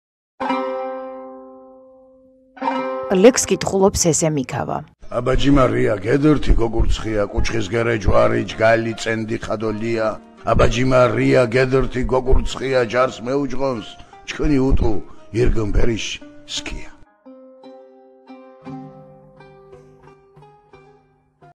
კოლხის კოლხური ლექსი 08.04.2015
ლექსს კითხულობს სესე მიქავა